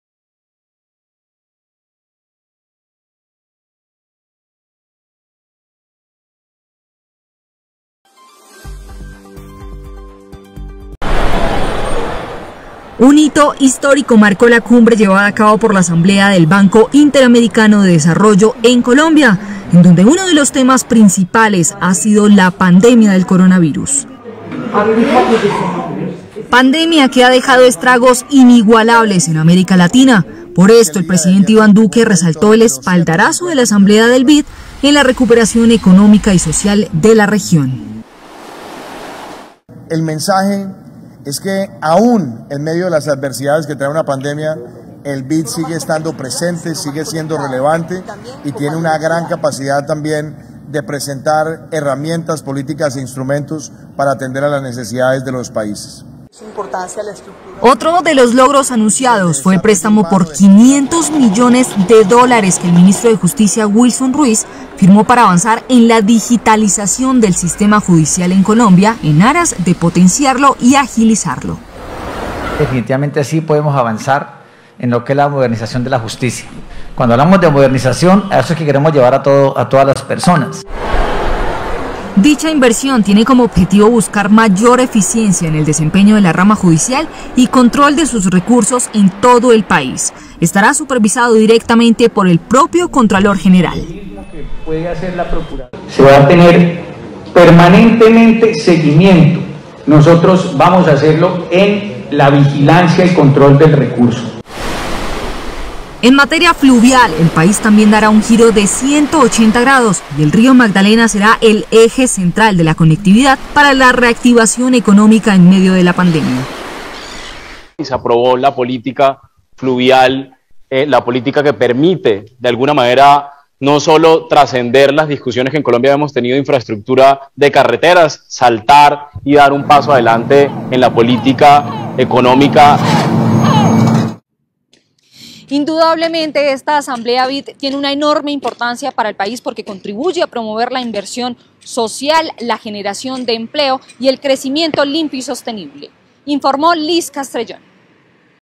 Las buenas noticias de la Asamblea del BID para la reactivación económica
VÍA: TELE HERÓICA /12:15